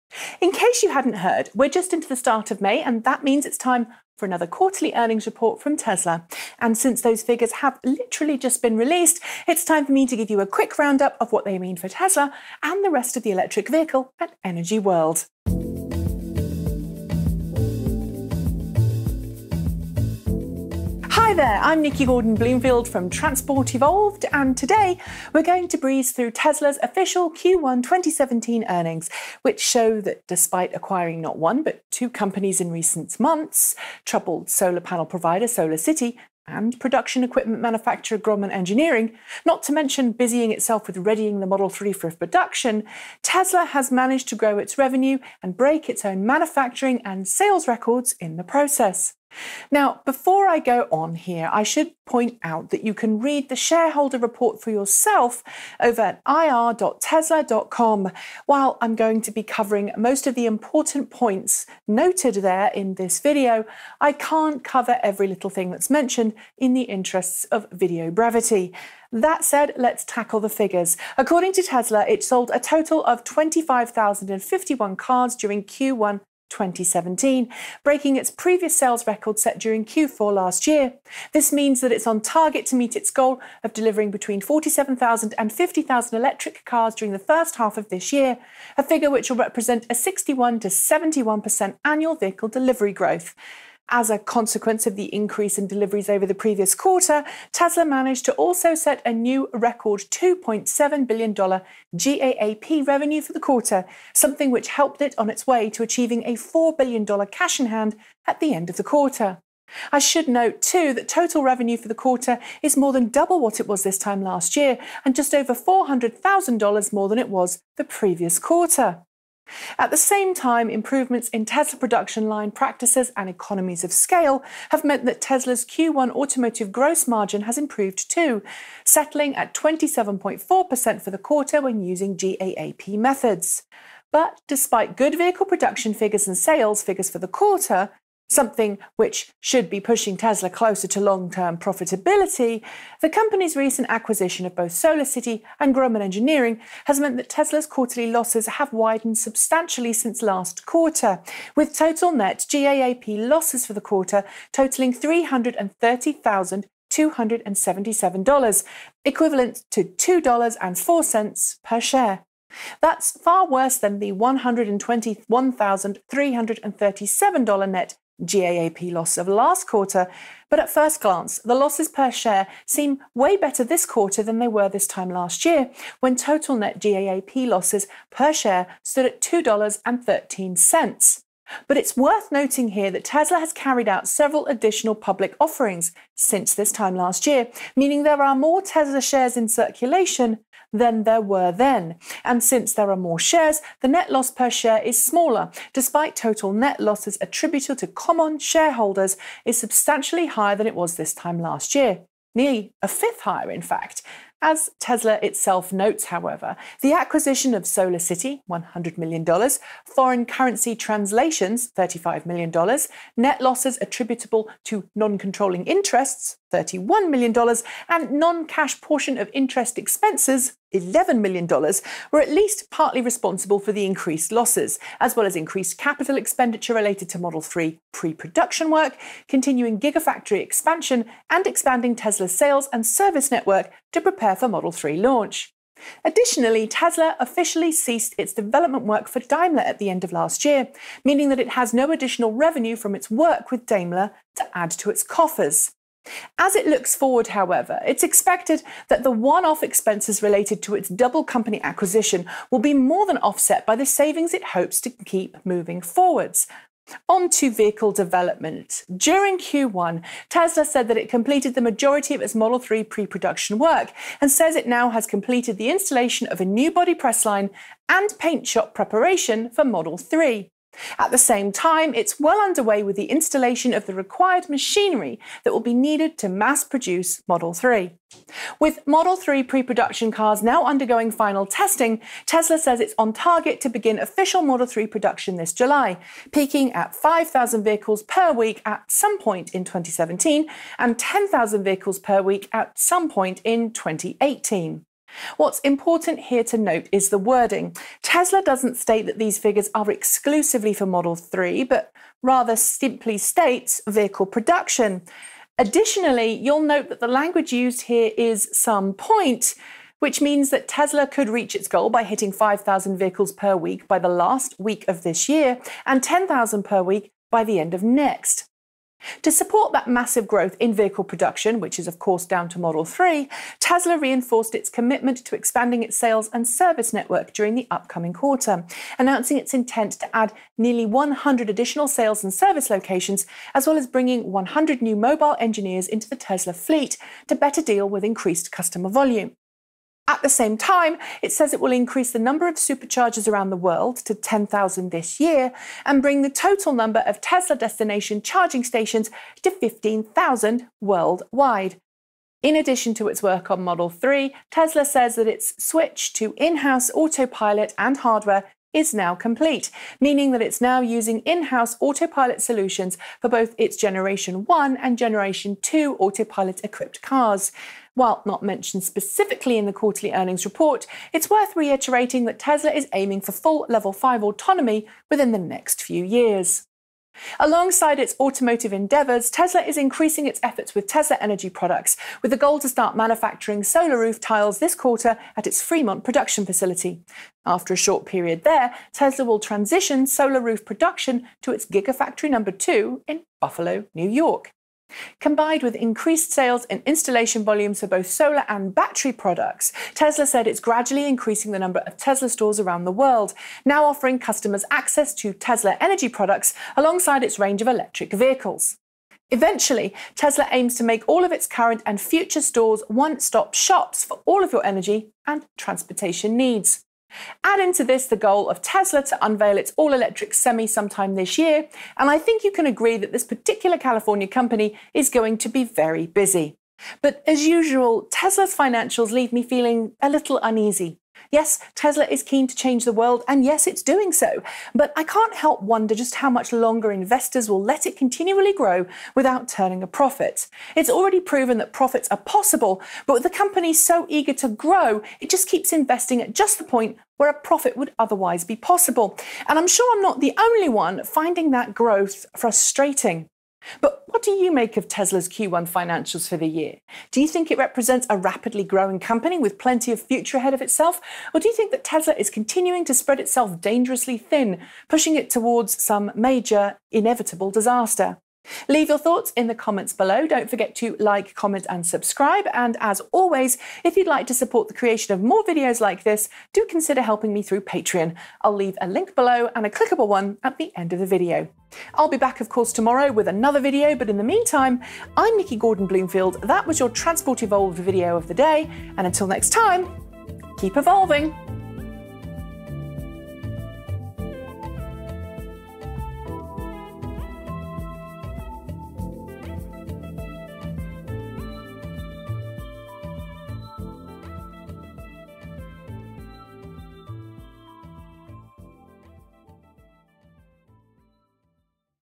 Tesla Q1 2017 Earnings Report -- The Important Bits
In case you hadn’t heard, we’re just into the start of May, and that means it’s time for another quarterly earnings report from Tesla.

And since those figures have literally just been released, it’s time for me to give you a quick roundup of what they mean for Tesla and the rest of the electric vehicle and energy world.

Watch the video above, and don't forget to leave your thoughts and Comments below.